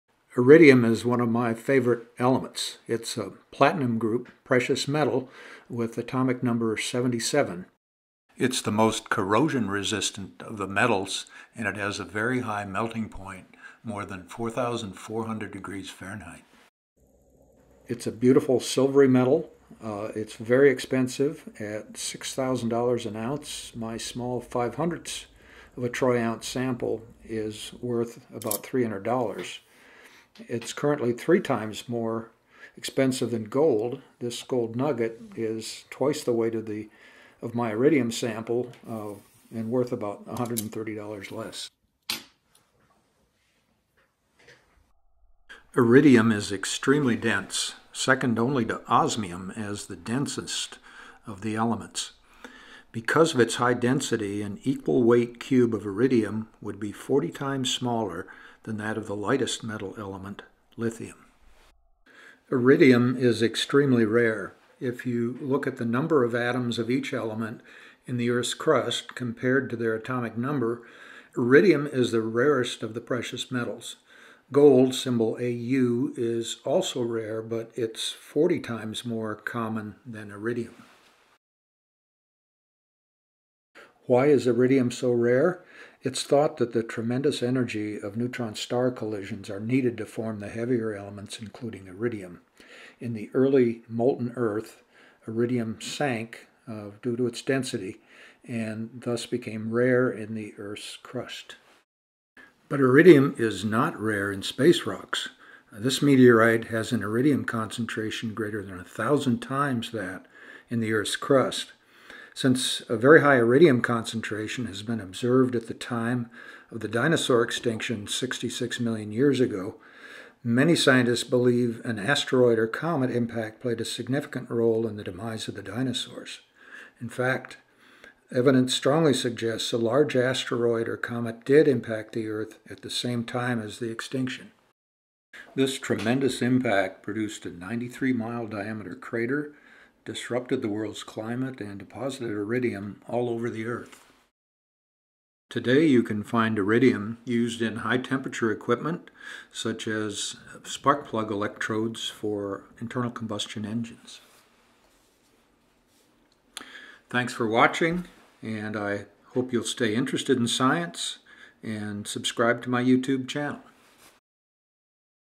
Iridium - The Element 40 Times as Rare as Gold
Description of the dinosaur extinction-related element Iridium. This extremely rare and very expensive precious metal is nearly as dense as the element Osmium.  It's relatively high concentration in space rocks led to the discovery of the Chicxulub impact crater.